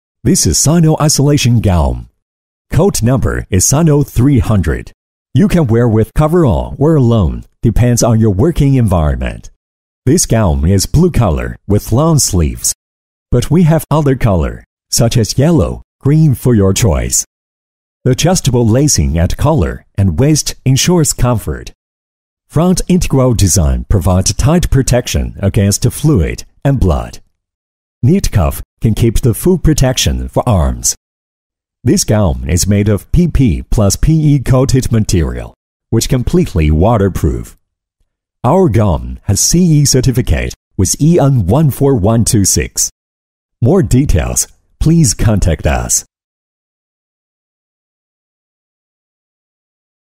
SINO UNION SUPPLY CHAIN (QINGDAO) CO.,LTD CORPORATE PUBLICITY VIDEO
This video is a promotional video of our company, showing the whole picture of the company in all directions, and showing in detail the whole process operation mode of our non-woven fabric disposable coverall  following cutting, sewing, packaging and transportation